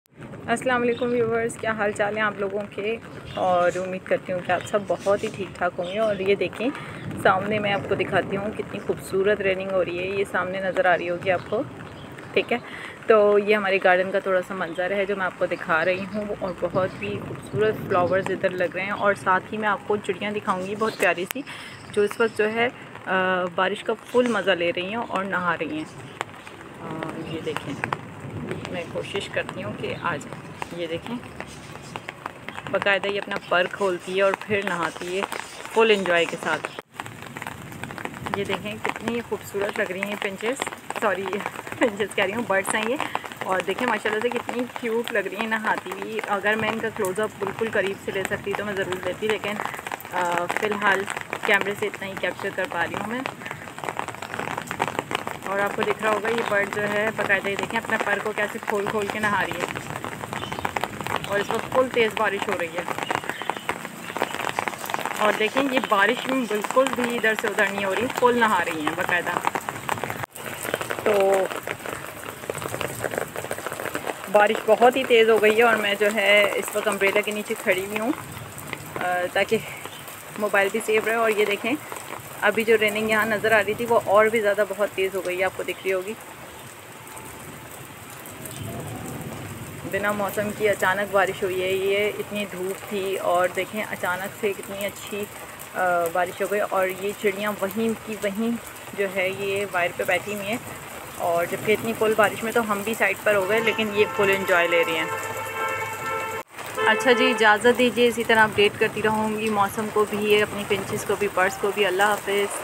heavy raining||birds enjoying weather||open wings in raining
heavy raining n birds are enjoying n taking bath happily garden look owsom